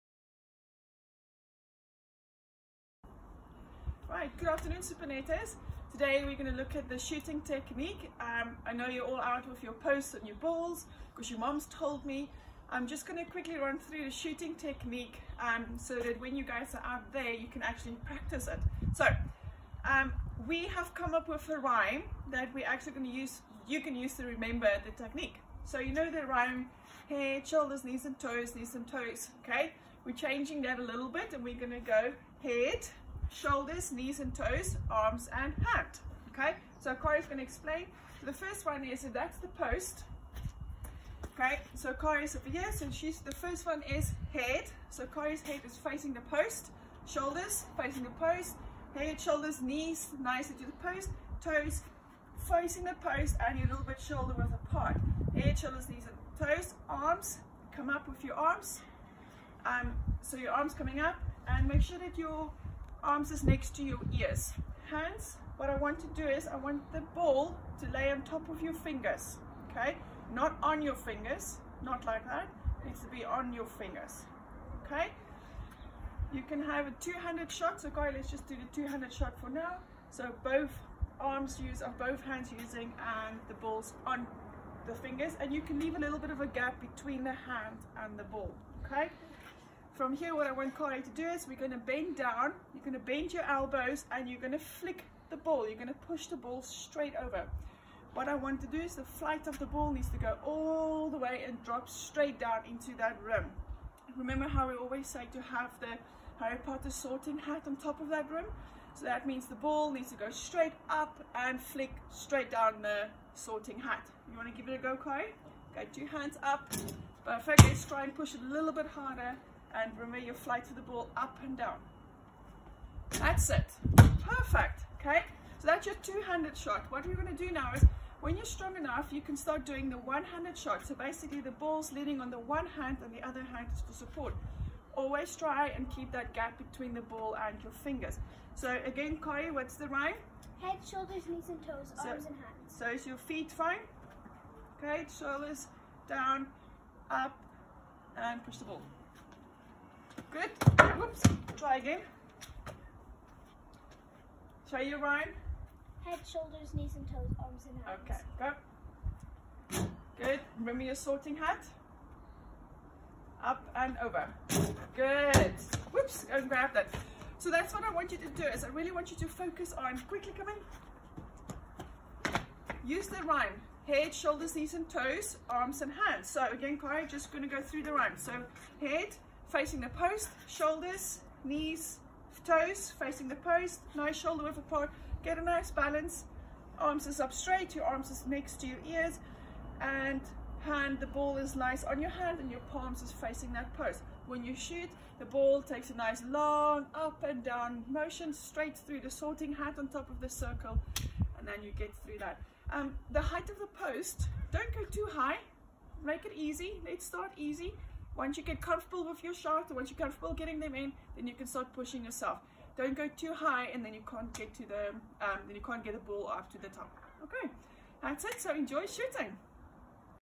SuperNetters - Shooting Technique
Easy explanation for our SuperNetters to see how to shoot.